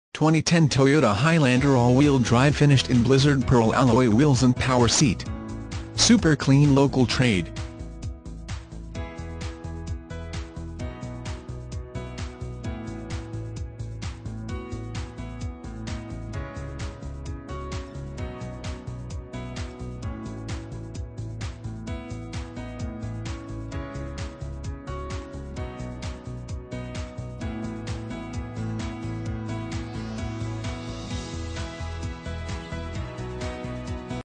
2010 Toyota Highlander - Sport Utility West Burlington IA 663422
2010 Toyota Highlander

2010 Toyota Highlander all wheel drive finished in Blizzard Pearl.Alloy wheels and power seat.Super clean local trade.